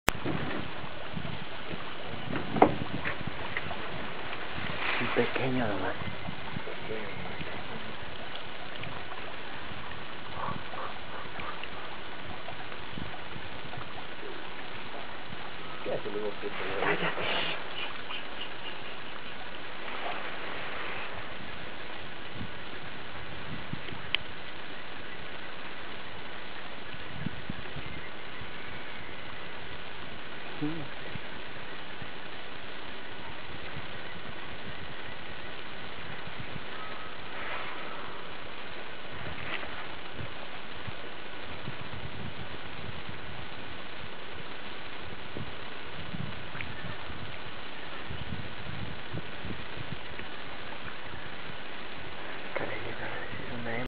Jaguar
A wild juvenile jaguar lazing of the river, seen from a lancha on the Rio Tiputini in Amazonian Ecuador.